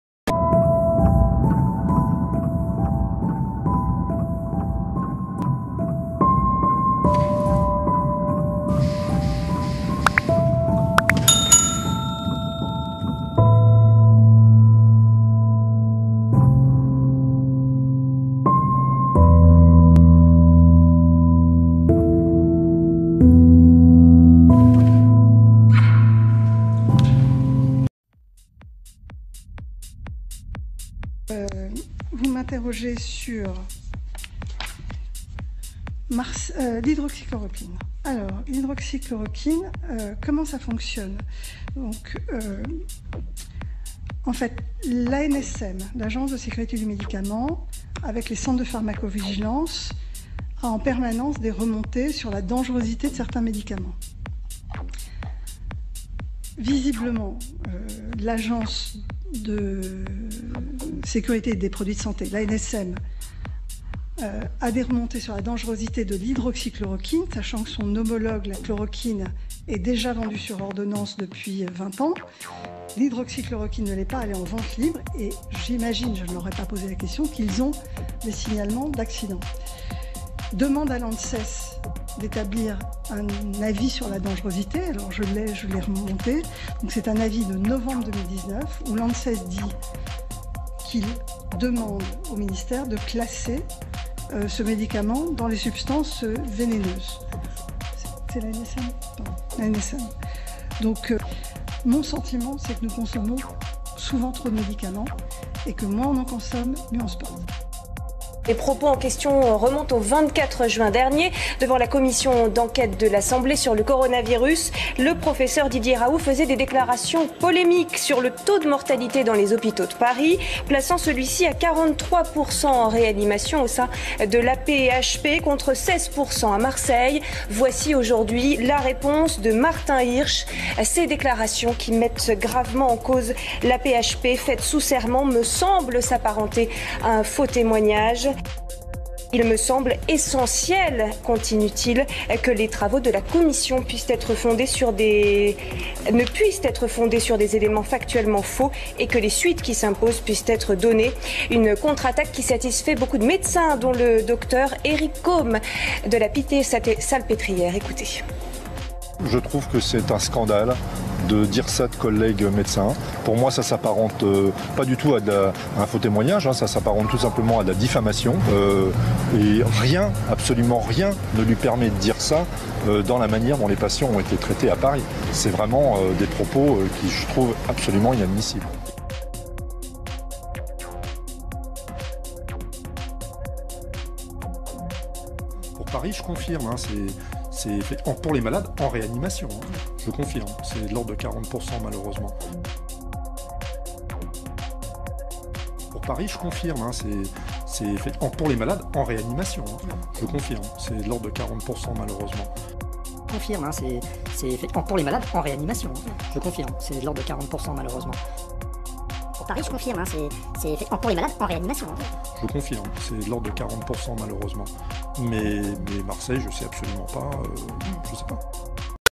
PROFESSEUR DIDIER RAOULT ACCUSÉ DE FAUX TÉMOIGNAGE PAR L'AP.HP
Les attaques contre le professeur Didier Raoult se sont multipliées ces derniers jours. Ce mercredi, le président de l'Assistance publique - Hôpitaux de Paris (AP-HP), Martin Hirsch, a transmis un courrier au président de l'Assemblée nationale, Richard Ferrand, dans lequel il accuse l'infectiologue marseillais d'avoir délivré un "faux témoignage" lors de son audition devant les députés de la commission d'enquête sur la gestion du Covid-19.
Quelques heures plus tard, le Conseil Scientifique et la Haute autorité de Santé ont pointé à leur tour du doigt le défenseur controversé de l'hydroxychloroquine comme traîtement pour le coronavirus, l'accusant de propos "infamants" et qui relèvaient de la "calomnie."
Jeudi dans la journée, sur son compte Twitter suivi par plus de 630.000 personnes, Didier Raoult a tenu à répondre à Martin Hirsch. Pour cela, il a partagé trois vidéos et articles publiés dans les médias au cours de ces dernières semaines, parfois au plus fort de la crise du Covid-19.
L'AP-HP dénonce plus particulièrement deux points de l'audition du Directeur général de l'IHU Méditerranée Infection et la communauté scientifique. Dans un premier temps, devant les députés, Didier Raoult avait assuré que la mortalité en réanimation était de 43% à Paris. "Chez nous elle est de 16%", avait-il insisté, présentant les statistiques personnelles de son institut.
Dans sa lettre, Martin Hirsch assure que "nous n'avons aucune donnée qui place à 43% la mortalité dans les réanimations de l'AP-HP", sans préciser à combien se monte ce taux. "D'autre part, il n'y a à ce jour aucune étude publiée qui analyse comparativement les taux de mortalité en réanimation, évaluées dans des conditions contrôlées, entre les hôpitaux parisiens et marseillais", ajoute-t-il.
Auprès de l'AFP, l'entourage du Pr Raoult a renvoyé à des résultats du registre Reva (Réseau européen de recherche en ventilation artificielle) figurant dans un "rapport de la cellule de crise de l'AP-HP du 14 avril". Dans ce document, qui date d'il y a deux mois et demi, période du pic de l'épidémie en France, le pourcentage de décès en réanimation était alors évalué à 43% à l'AP-HP et 41% hors AP-HP.
Les deux premiers documents de son triptyque abordent justement ce point. Dans un premier temps, il a mis en ligne un extrait d'une interview donné fin juin par le Pr Éric Caumes, chef du service des maladies infectieuses à l’hôpital de la Pitié-Salpêtrière à Paris, établissement de l'AP-HP, dans laquelle ce dernier évoque les chiffres parisiens.
Pour Paris je confirme, pour les malades en réanimation, c’est de l’ordre de 40% malheureusement, mais Marseille je ne sais absolument pas", affirmait-il à LCI.
Quels patients en réanimation?
Puis, l'infectiologue s'est également appuyé sur les déclarations d'une de ses principales opposantes, le Dr Karine Lacombe. La cheffe du service des maladies infectieuses à l’hôpital Saint-Antoine à Paris, établissement de l'AP-HP, évoquait mi-avril sur BFMTV l'évolution des patients atteints du coronavirus au sein de son établissement.Certains patients se dégradent en salle de médecine et doivent passer en réanimation. Ce n’est jamais
Or, et c'est l'un des principaux reproches faits à l'encontre de Didier Raoult par le corps scientifique, si ces chiffres sont bel et bien évoqués, ils ne prennent pas en compte la gravité de l'état de santé des malades pris en charge par les services hospitaliers parisiens. Des critiques auxquelles l'infectiologue n'a pas répondu sur Twitter.

"Parler de mortalité brute n’a aucun sens puisque la mortalité est liée à la sévérité des malades", rétorquait Antoine Vieillard Baron, anesthésiste réanimateur à l’hôpital Ambroise-Paré de Boulogne-Billancourt, sur BFMTV. "Plus les malades sont admis dans des cas sévères, plus leur mortalité attendue va être élevée."
"Les données épidémiologiques, notamment de mortalité en réanimation en Île-de-France ne sont pas connues, ne sont pas publiées. Toutes nos données ont été mises dans un registre nommé ‘Covid ICU’ qui est dirigé par le Pr Schmidt à l’hôpital de la Pitié-Salpêtrière, mais elles ne sont pas publiées. Il faut attendre la fin de l’épidémie pour donner un chiffre de façon fiable", rappelait ce mercredi, toujours sur BFMTV, Bruno Megarbane, chef du service de réanimation médicale à l’hôpital Lariboisière de Paris, un établissement de l'AP-HP.

Un cas non détecté?
Deuxième sujet évoqué, celui du premier patient atteint du Covid-19 à avoir trouvé la mort sur le sol français. Pour Didier Raoult, ce dernier n'a pas été pris en charge de manière correcte.